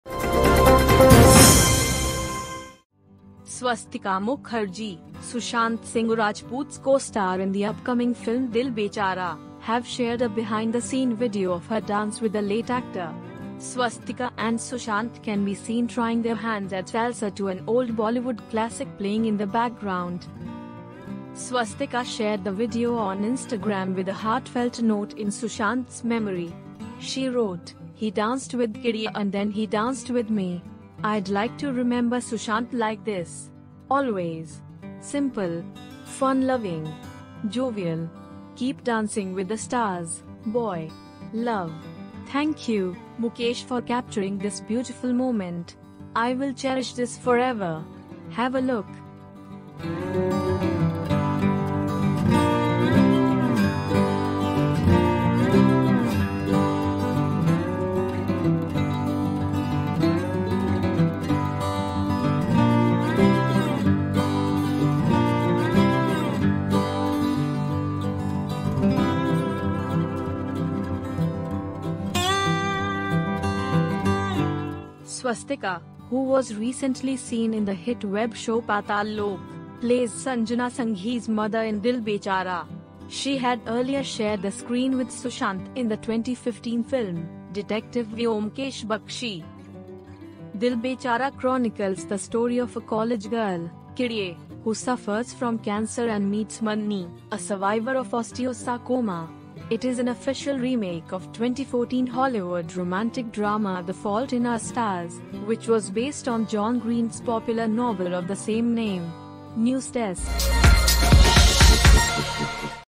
Watch, Swastika Mukherjee shares unseen dance video with Sushant Singh Rajput
Swastika Mukherjee, Sushant Singh Rajput’s co-star in the upcoming film Dil Bechara, have shared a behind the scene video of her dance with the late actor. Swastika and Sushant can be seen trying their hands at salsa to an old Bollywood classic playing in the background.